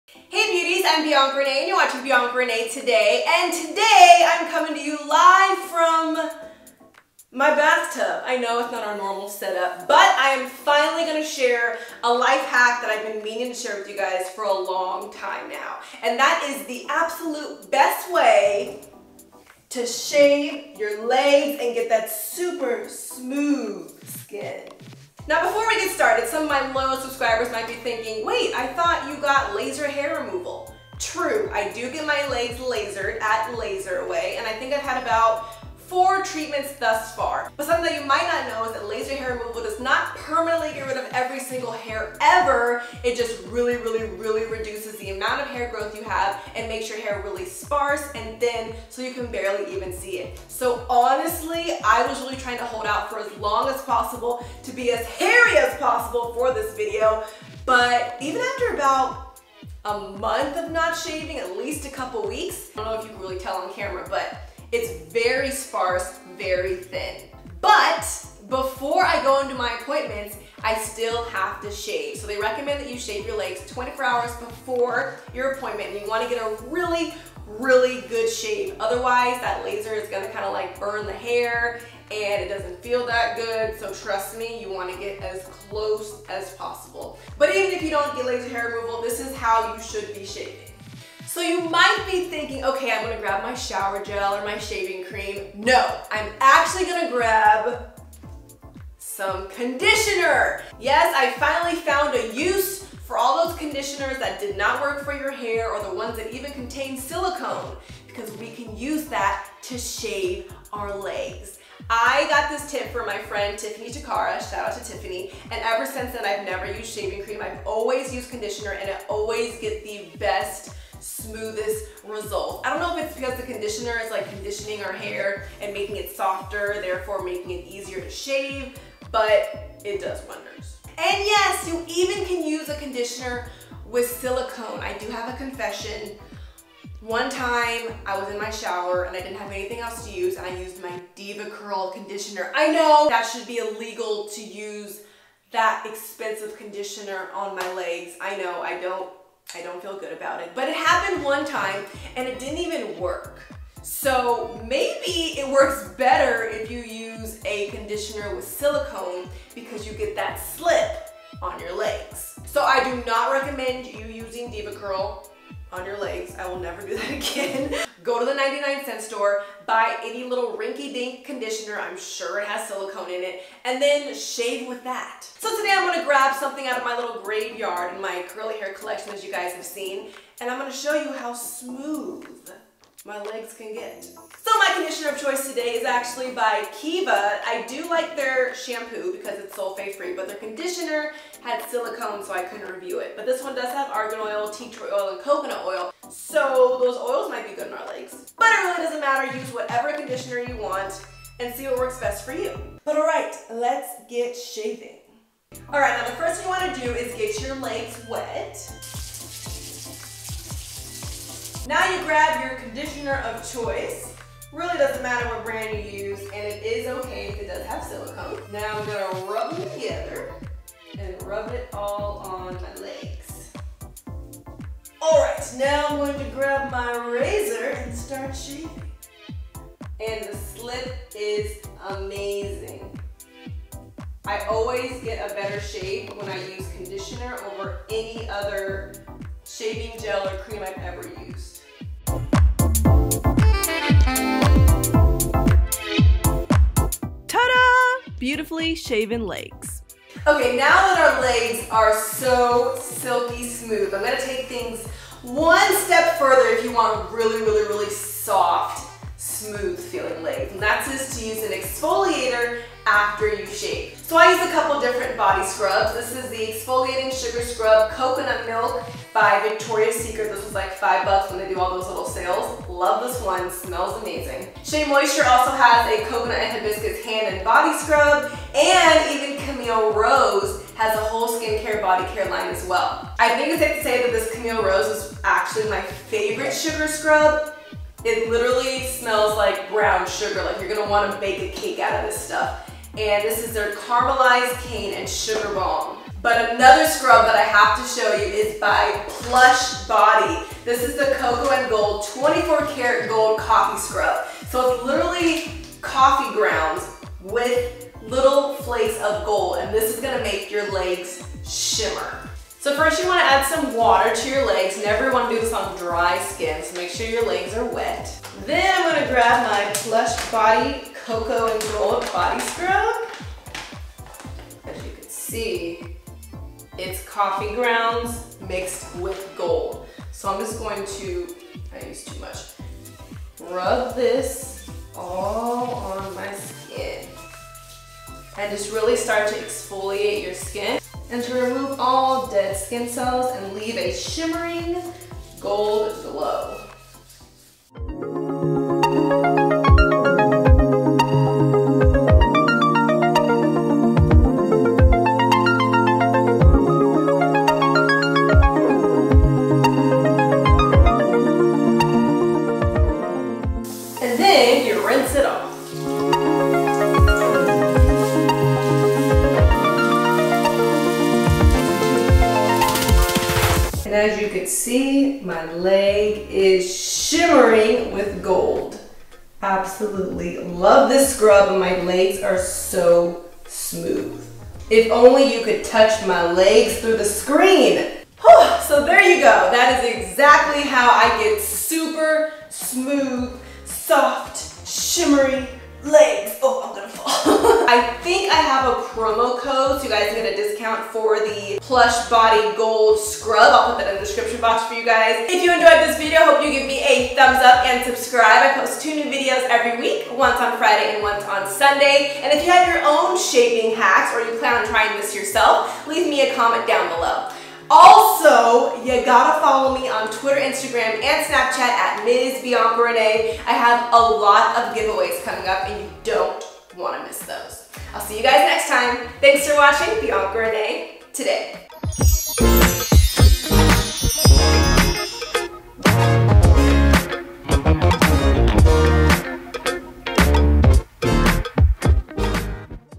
Shaving With Conditioner! LifeHack + Laser Hair Removal Update! | BiancaReneeToday
The BEST way to shave your legs! Ever since I started using conditioner to shave, I have gotten super smooth results. Exfoliating after also makes your skin really soft. Give it a try and let me know how it works for you!

I get my Laser Hair Removal treatments done at @LaserAway in Woodland Hills California. to get more information on locations, pricing and services!

Conditioner to use: ANY conditioner. This is what I do with all of the conditioners my hair doesn't like, even if it has silicone. Feel free to try whatever you have in your bathroom!

Body Scrubs mentioned:
Camille Rose Naturals
Shea Moisture
Victoria's Secret